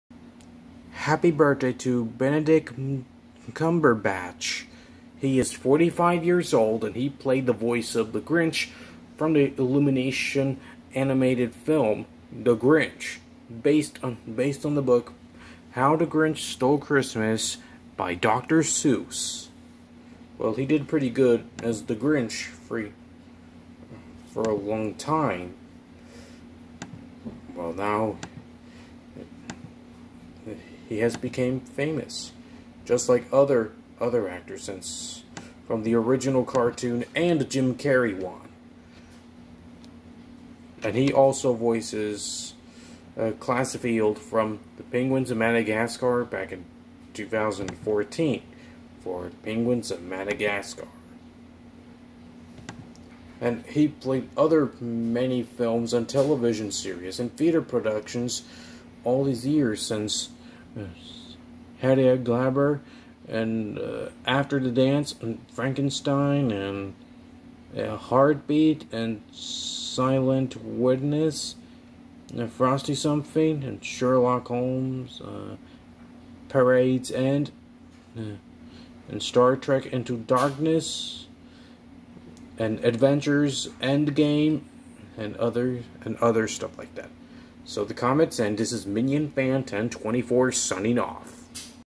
Happy 45th Birthday to Benedict Cumberbatch!
He voices The Grinch from Illumination and Dr. Seuss "The Grinch".